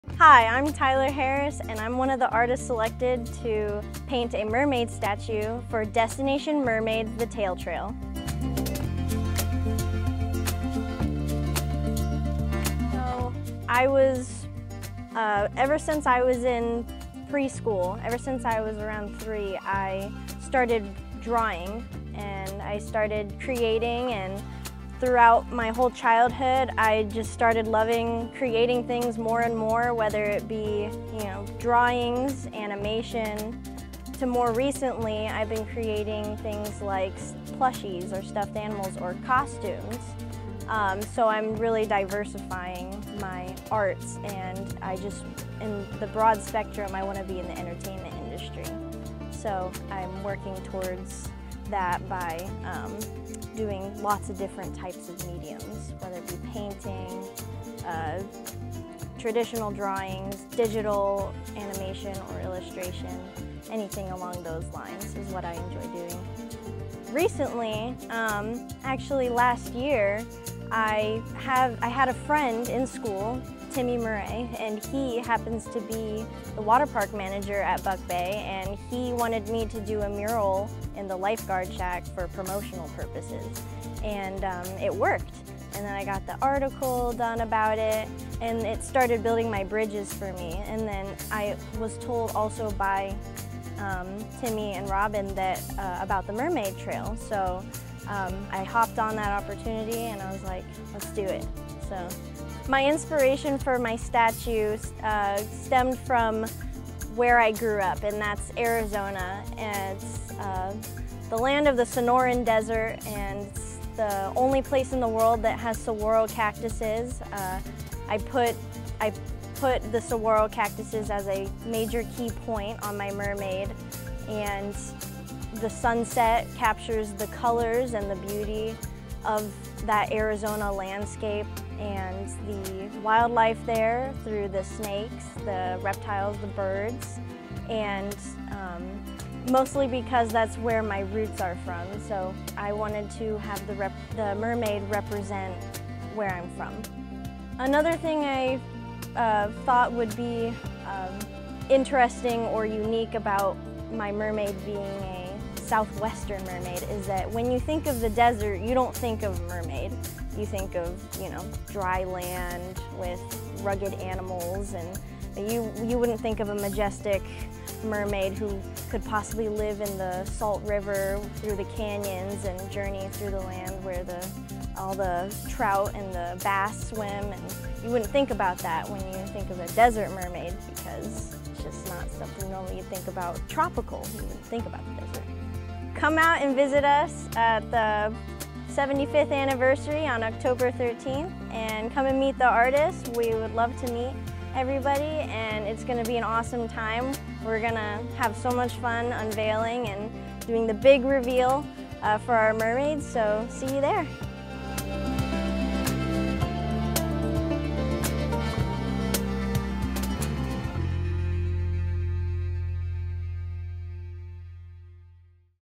Meet the Artist: Tyler Harris for Destination Mermaids - The Tail Trail (2022)
A video I created for the Destination Mermaids - The Tail Trail series. This video is with Artist, Tyler Harris.